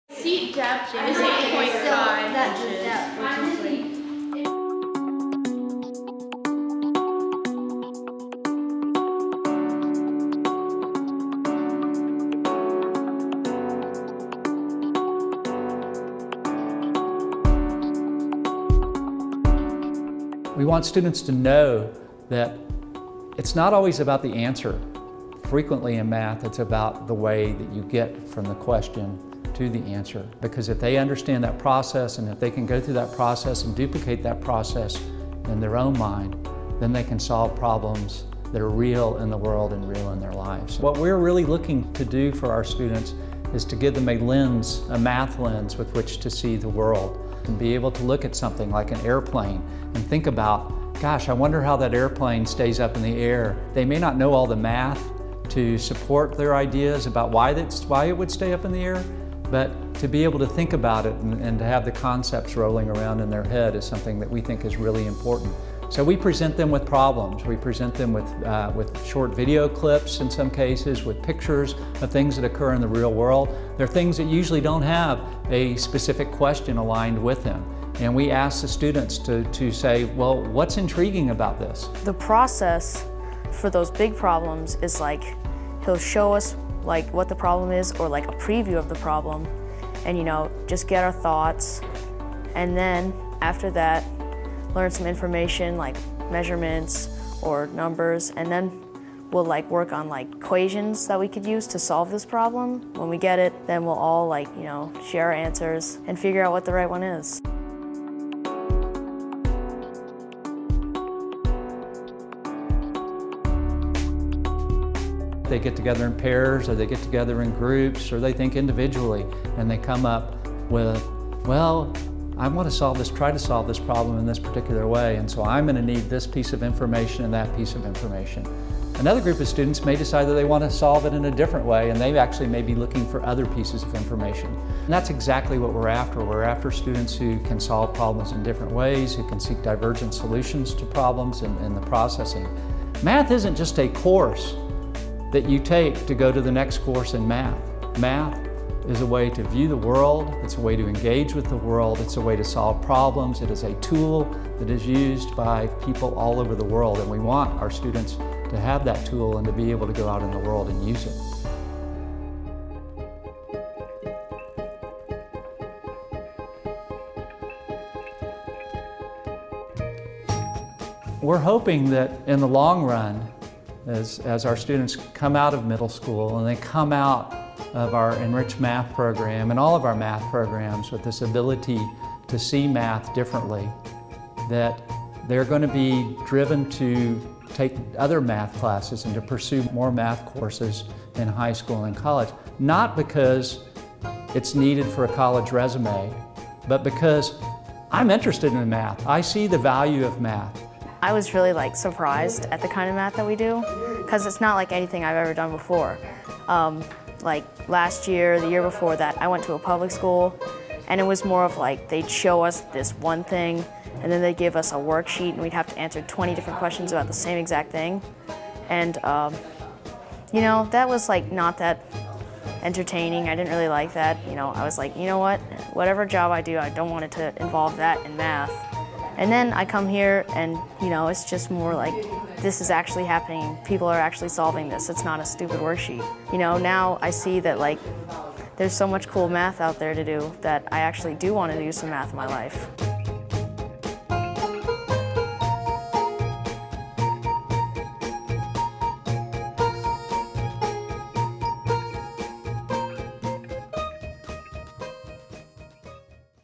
Middle School Math
90% of US students are not interested in pursuing careers in applied math. Carolina Day School's curriculum shows students how solving equations can be meaningful in their lives and, as a result, inspires them to learn the material. This gives them a "math lens" with which to see the world, not just a class to complete.